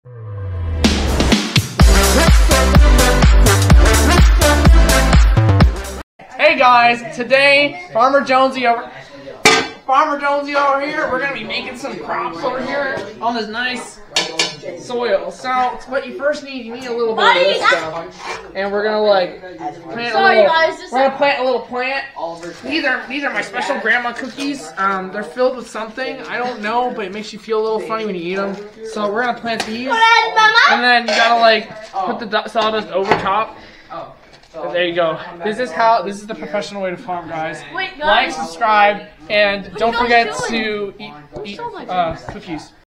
Farm'in with farmer Jonesy 🚜
Me and the boys somehow find a way to farm on concrete featuring famous farmer Jonesy.